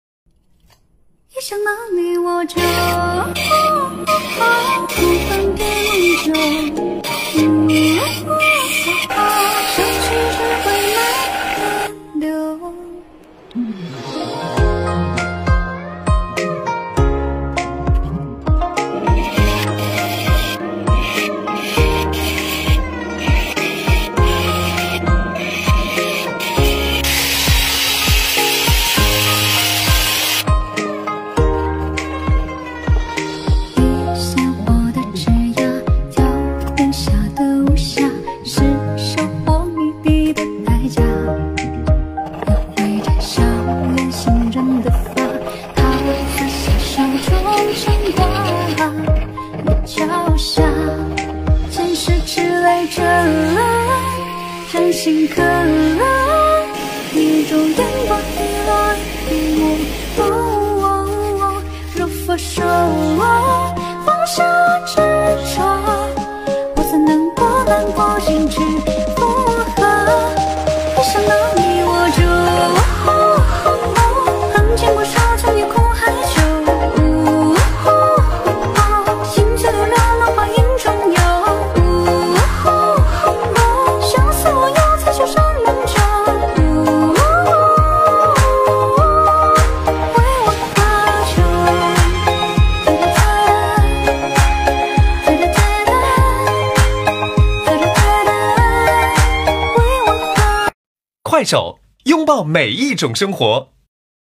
The birth process of a nice fork
Don't throw away any natural wooden fork, because it can be turned into a beautiful slingshot,lol. To view more wonderful slingshots